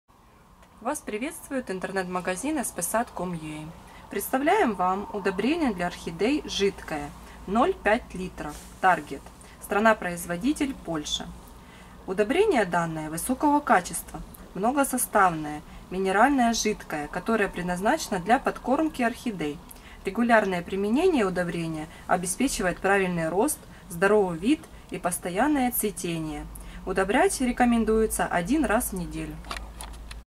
Удобрение для ОРХИДЕЙ 0,5л TARGET жидкое
Высокого качества, многосоставное, минеральное жидкое удобрение, которое предназначено для подкормки орхидей.